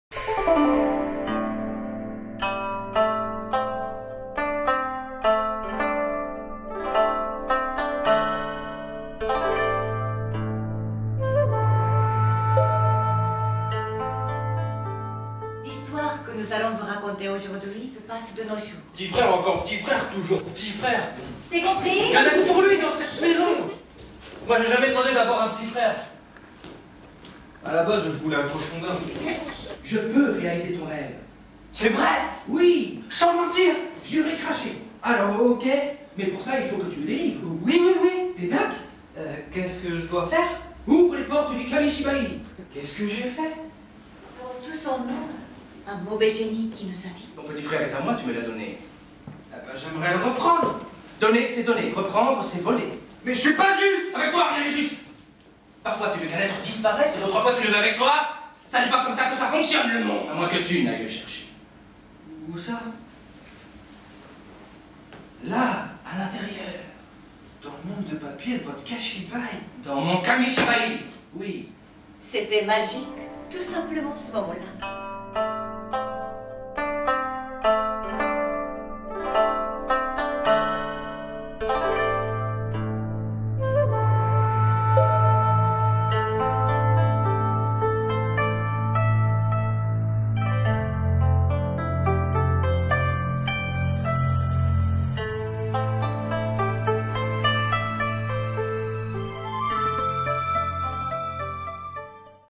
KAMISHIBAI (teaser/vingtième théâtre)
Spectacle KAMISHIBAÏ.
Ecrit et mis en scène F. Arca.
Avec Hiromi Asaï, Emmanuel Bidet, Nicolas Jauregui et Alexandra Jussiau.
A partir du 29 Janvier 2014 au Vingtième Théâtre. Les mercredis à 14h30 et pendant les vacances scolaires.